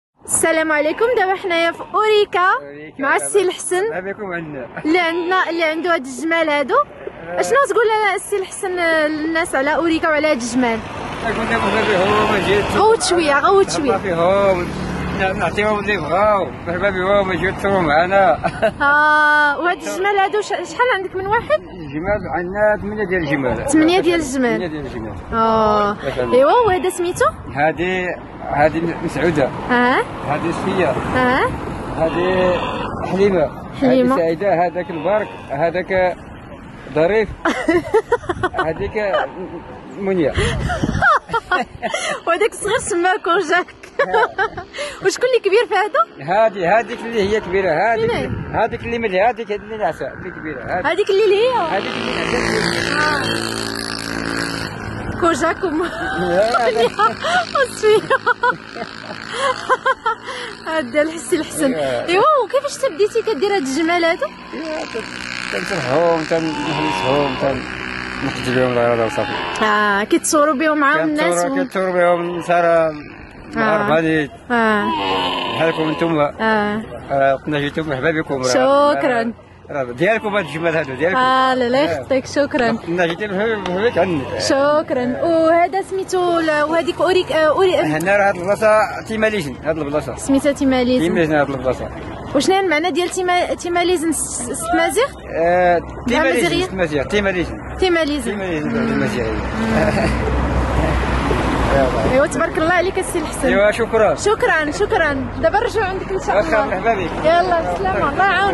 البدو من الصحراء | Nomads of the Sahara
Lahcen lives in Ourika and has eight camels. Each camel has his unique name | لحسن يعيش في أوريكا ولديه ثمانية جمال. كل جمل له اسم فريد